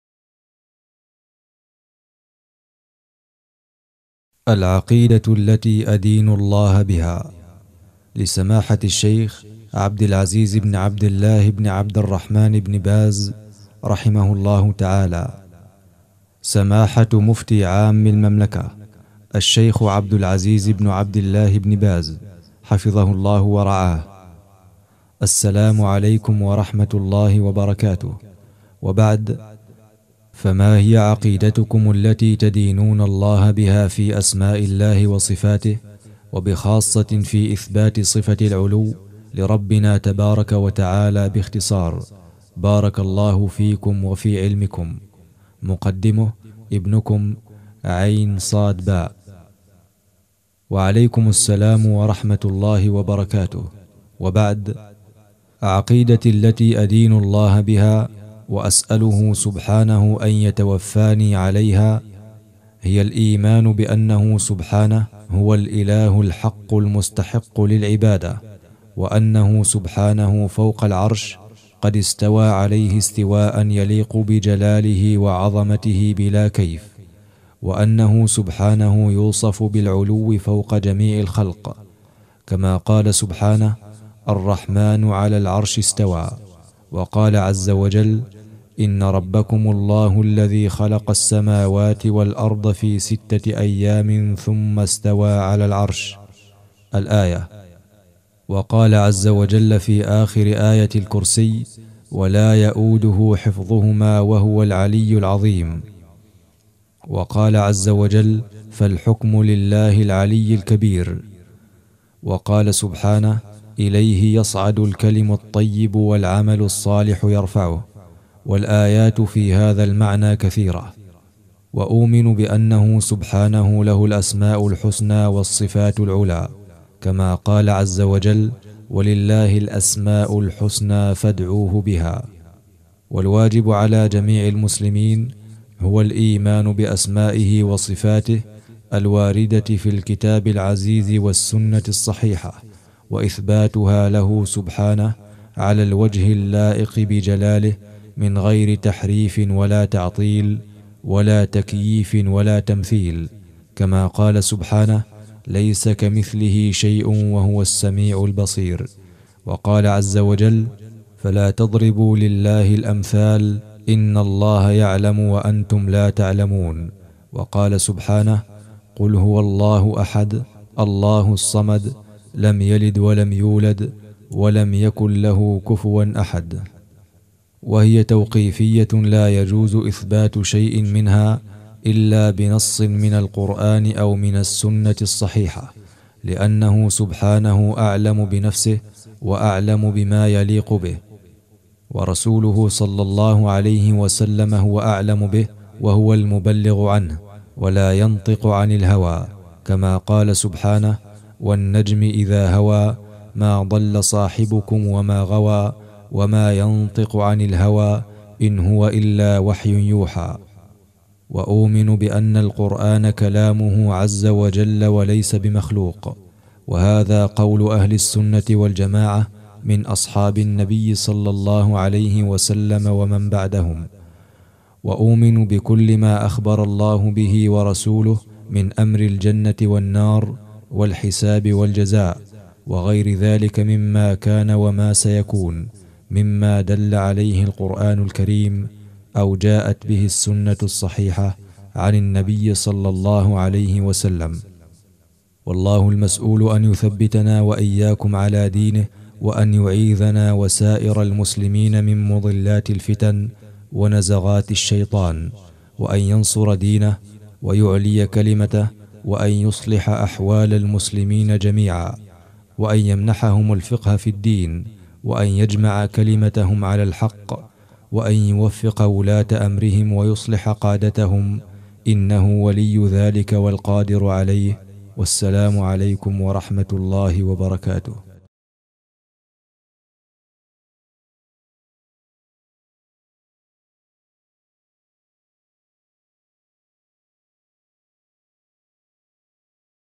La croyance par laquelle je voue un culte à Allah - Sheikh Ibn Baz
Pour un meilleur visionnage veuillez mettre la vidéo en ᴴᴰ.

Lu par le frère Aboû Ahmad Ash-Shaydzmi qu'Allah le récompense !